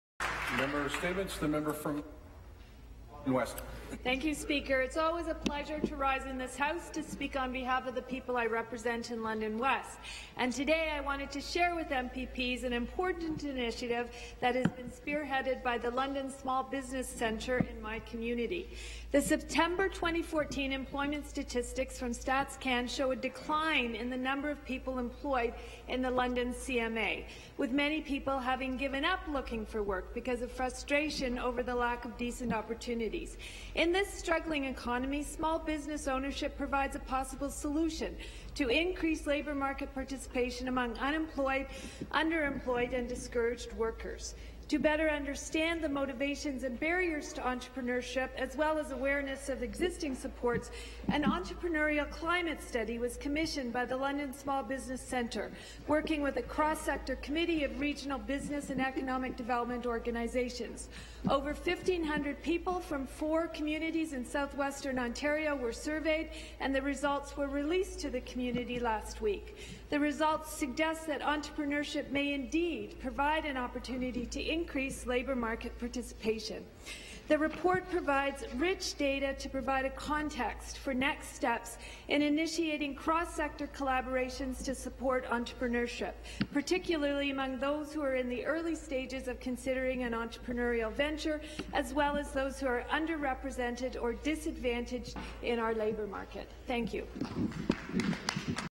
Small Business
It’s always a pleasure to rise in this House to speak on behalf of the people who I represent in London West.

Today, I wanted to share with MPPs an important initiative that has been spearheaded by the London Small Business Centre in my community.

The September 2014 employment statistics from StatsCan show a decline in the number of people employed in the London CMA with many people having given up looking for work because of frustration over the lack of decent opportunities.

The results suggest that entrepreneurship may indeed provide an opportunity to increase labour market participation. The report provides rich data to provide a context for next steps in initiating cross-sector collaborations to support entrepreneurship, particularly among those who are in the early stages of considering an entrepreneurial venture, as well as those who are underrepresented or disadvantaged in our labour market.

In this struggling economy, small business ownership provides a possible solution to increase labour market participation among unemployed, underemployed and discouraged workers.

To better understand the motivations and barriers to entrepreneurship, as well as awareness of existing supports, an entrepreneurial climate study was commissioned by the London Small Business Centre working with a cross-sector committee of regional business and economic development organizations. Over 1,500 people from four communities in southwestern Ontario were surveyed and the results were released to the community last week.